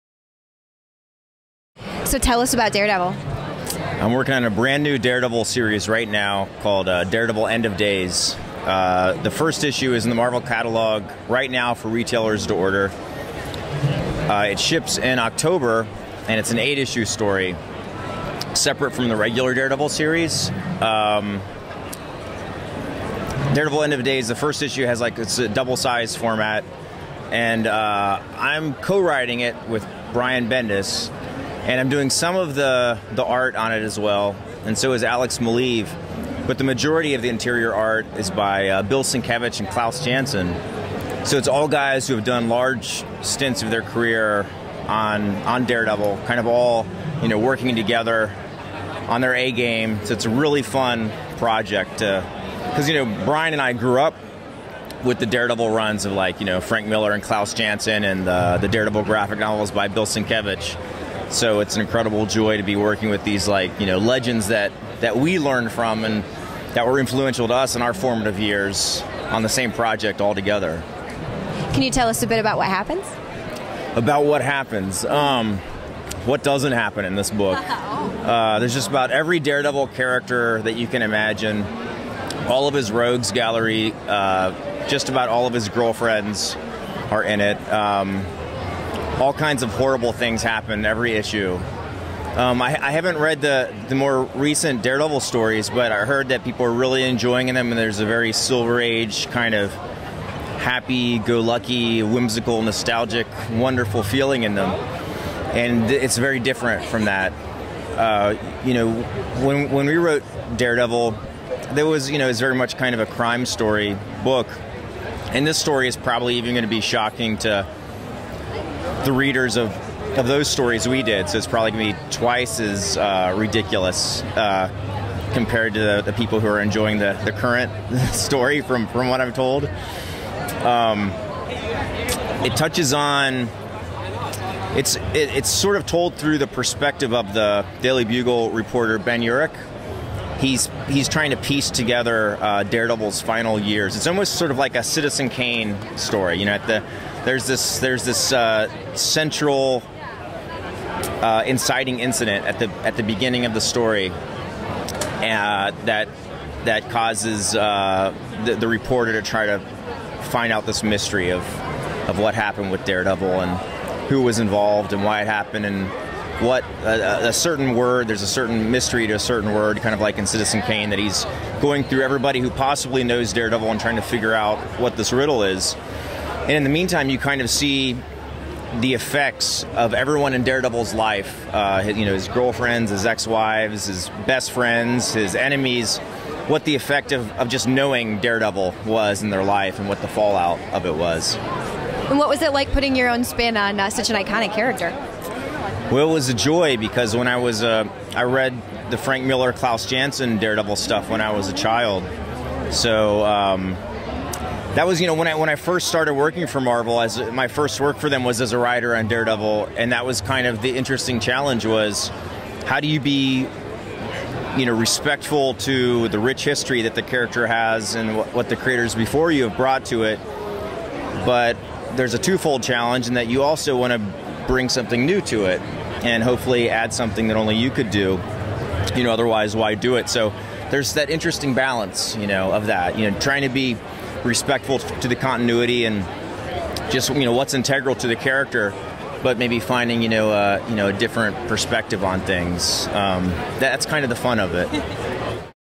David Mack talks Daredevil: End of Days
David Mack talks Daredevil: End of Days in longer interview from Comics Continuum TV show